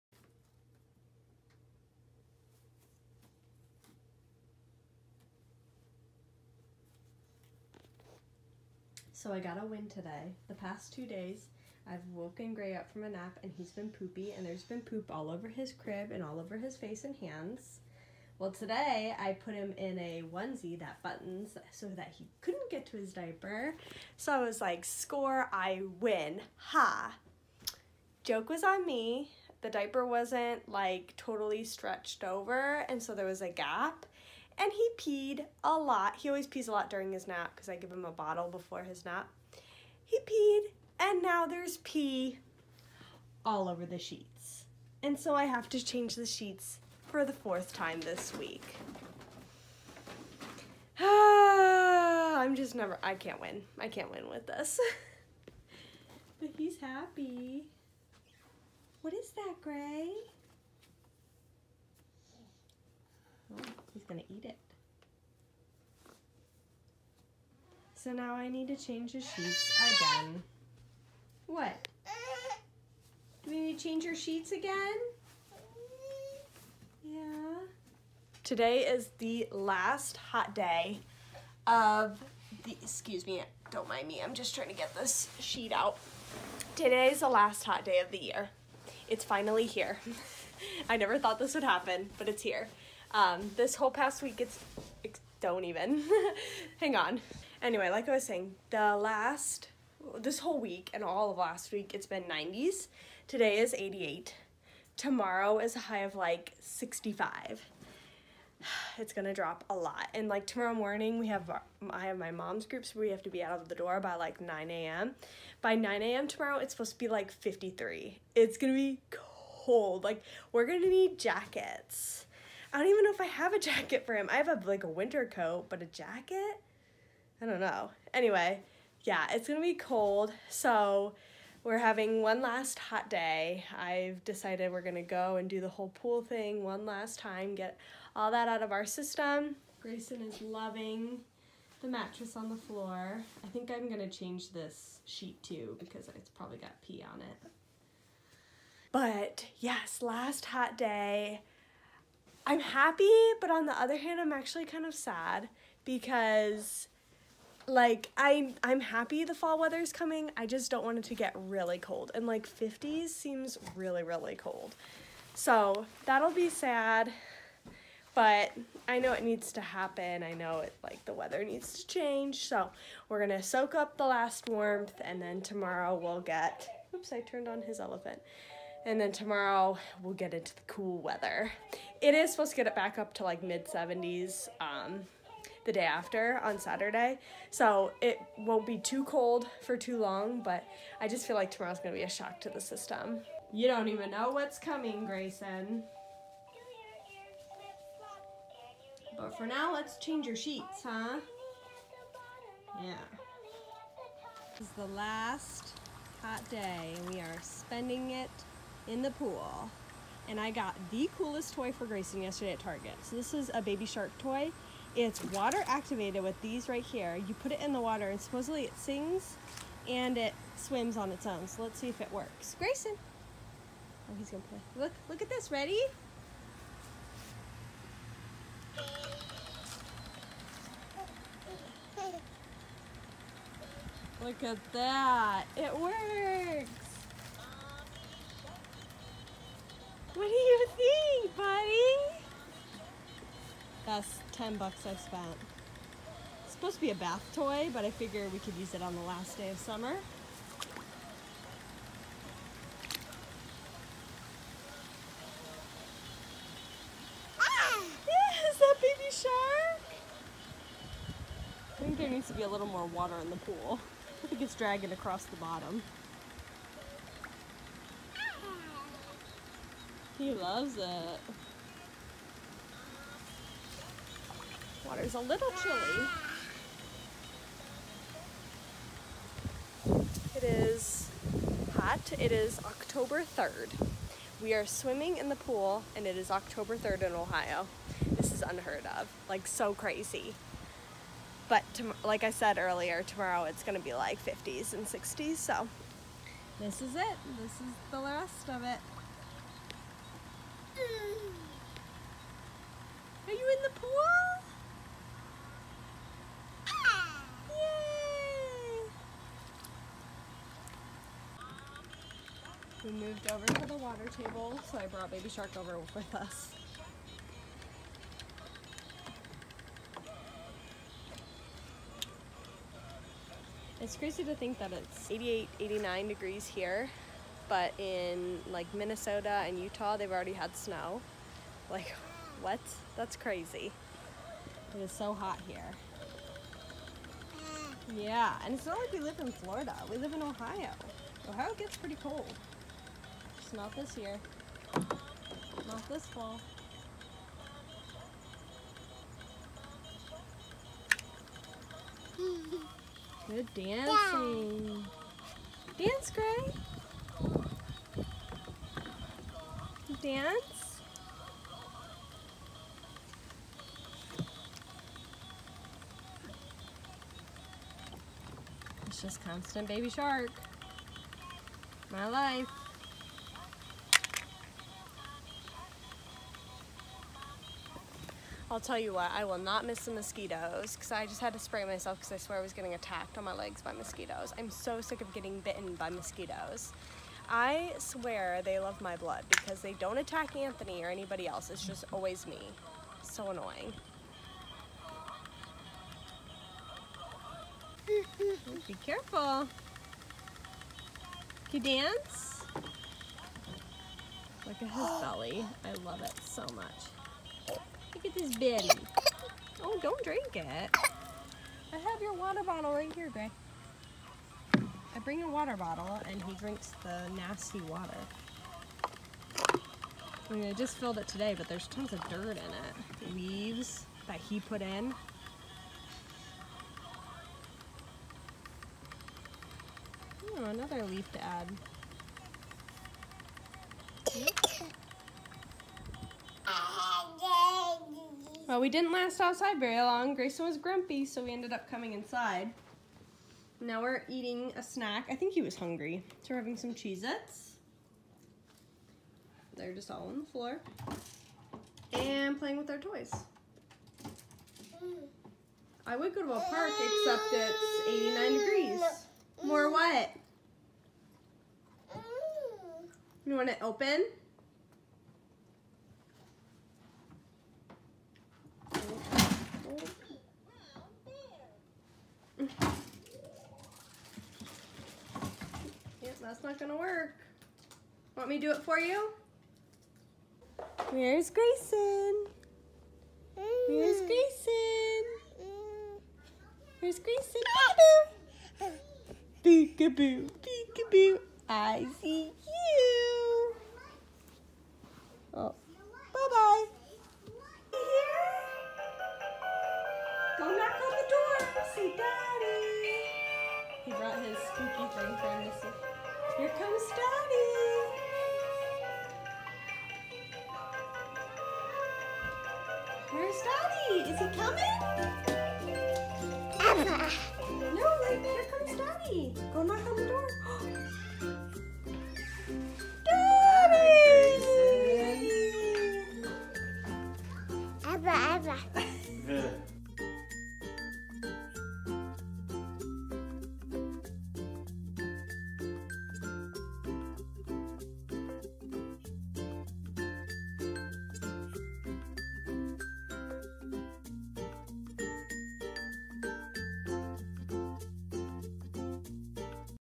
The Best 10 Dollars I Have Ever Spent!
Shannon deals with another poop incident and she tells you about the best 10 dollars that she has ever spent!